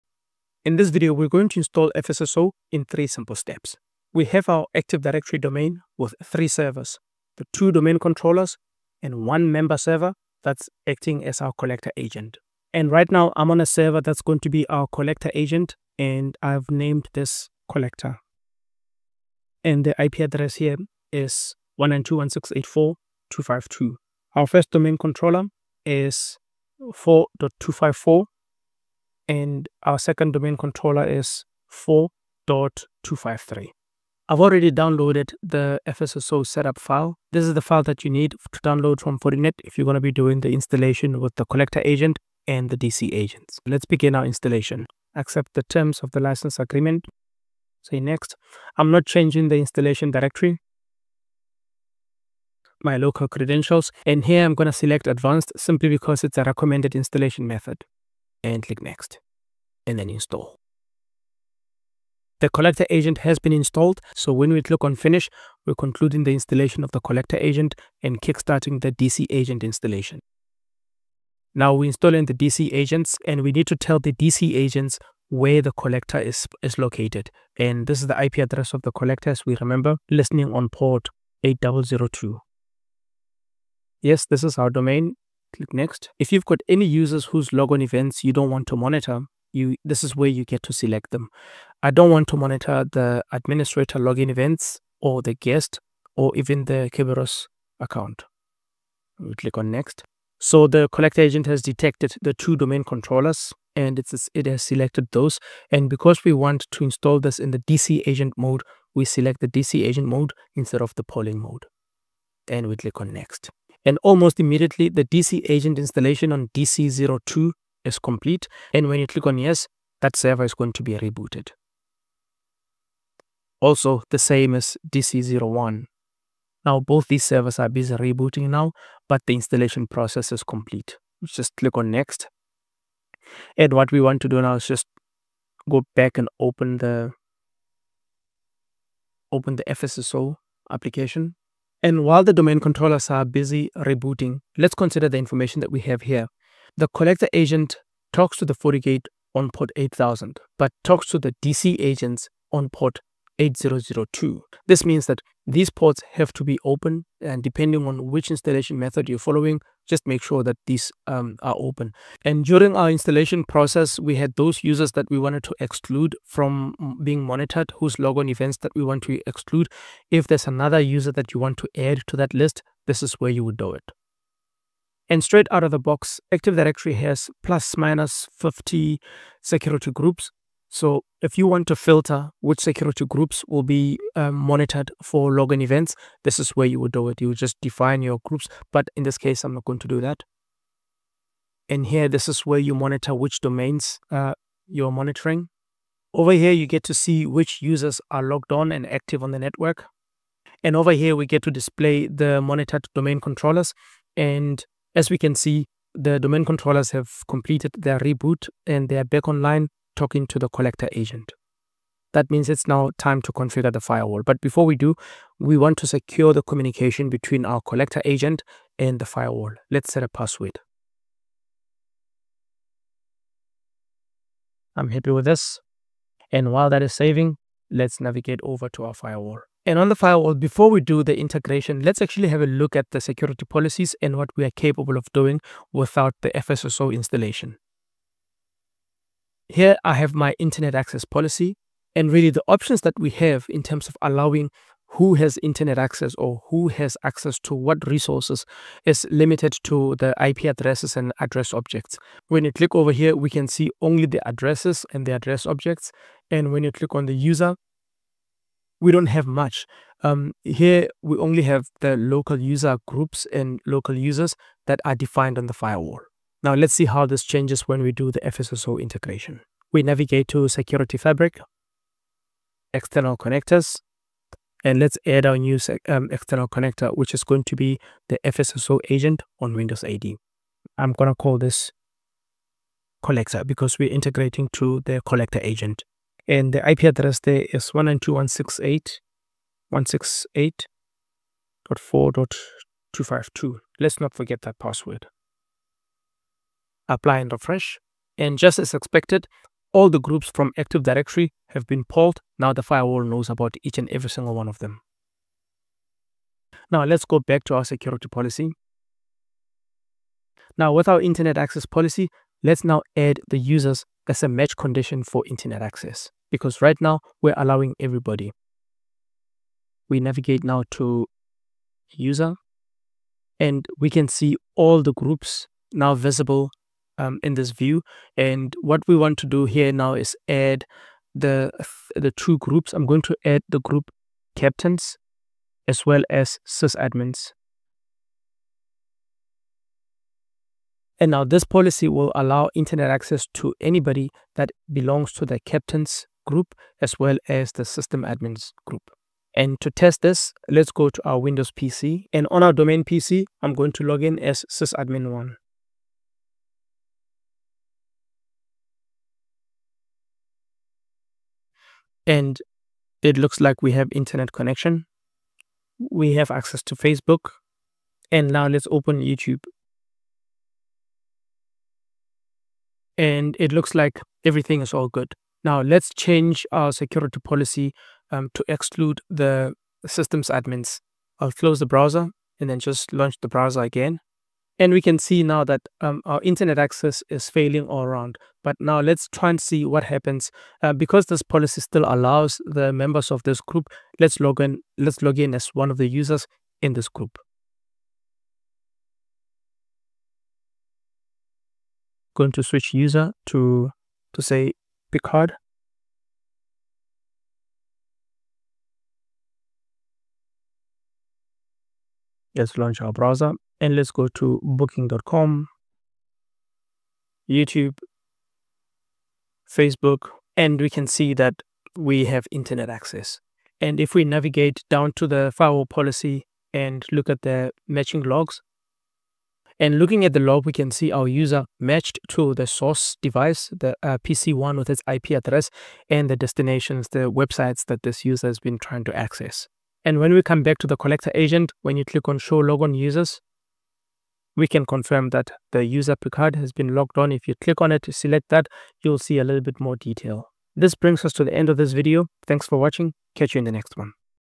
Fortigate: How to Install FSSO Agent with Collector in 3 EASY Steps!
Are you ready to enhance your network security with Fortinet FSSO? In this concise video, we’ll simplify the complexities of Fortinet’s Single Sign-On (SSO) solution and guide you through its efficient implementation in your organization.

You’ll learn everything from the basic architecture to configuring FSSO policies, empowering you to deploy and manage Fortinet FSSO like a pro! Ideal for network administrators, security engineers, and IT managers, this video is your key to a more streamlined network security infrastructure.

Watch now and take the essential step toward a more secure network!

**Timestamps:**
00:00 - Introduction & Lab Setup
00:44 - Step 1: Install & Configure FSSO Collector & DC Agents
03:50 - Step 2: Fortigate: Setup External Connector
05:06 - Step 3: Configure Firewall Policy & Validate!